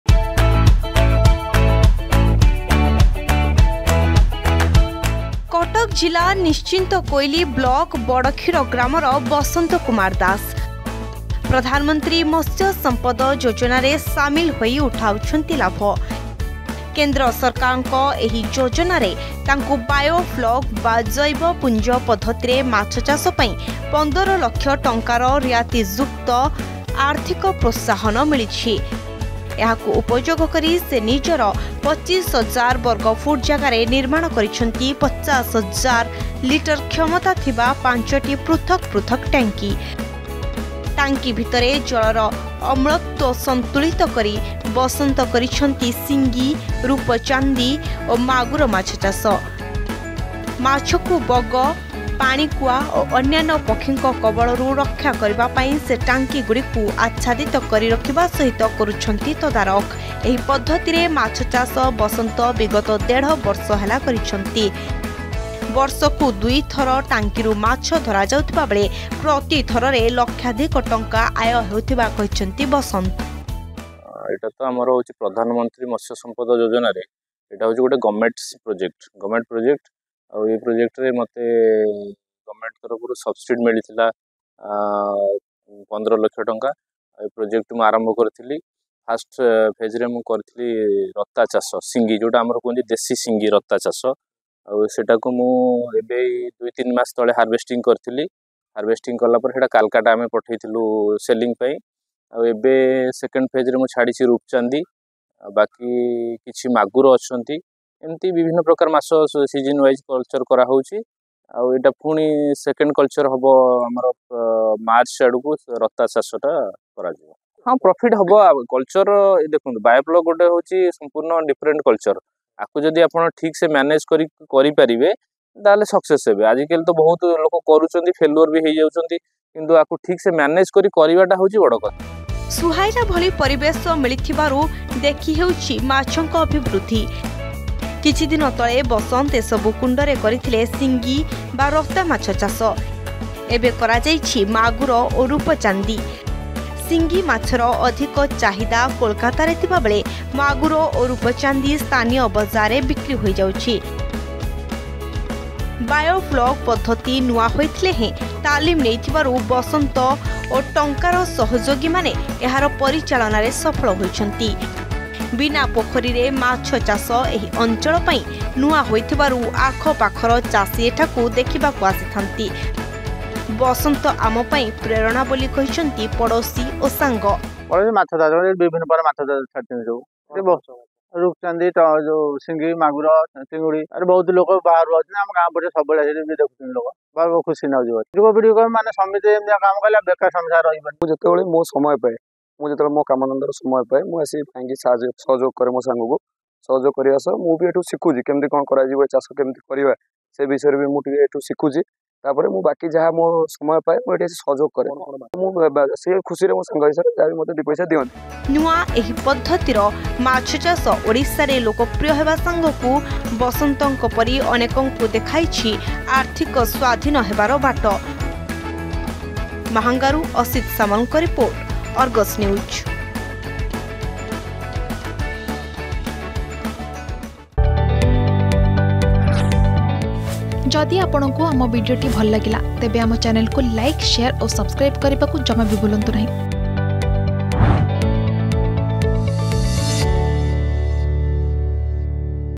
ଅଧିକ ଆୟ ଦେଉଛି କୁଣ୍ଡରେ ମାଛଚାଷ || Biofloc Fish Farming || GoodNews
ମାଛଚାଷ ପାଇଁ ଆଉ ଲୋଡ଼ାପଡ଼ୁନି ପୋଖରୀ । ବାୟୋଫ୍ଲକ ପଦ୍ଧତିରେ କୁଣ୍ଡରେ ହେଉଛି ମାଛଚାଷ । ଏହା ପାରମ୍ପରିକ ମାଛଚାଷ ଠାରୁ କମ ଜାଗା ଓ କମ ଖର୍ଚ୍ଚରେ କରିହେଉଛି । ଏଭଳି ପଦ୍ଧତି ଆପଣାଇ କଟକ ଜିଲ୍ଲା ନିଶ୍ଚିନ୍ତକୋଇଲି ବ୍ଲକ ବଡକ୍ଷୀର ଗାଁର ଯୁବଚାଷୀ ବସନ୍ତ କୁମାର ଦାସ ବର୍ଷକୁ ଦୁଇଥର ମାଛ ଅମଳକରି ଲକ୍ଷାଧିକ ଟଙ୍କାର ଲାଭ ପାଉଛନ୍ତି ।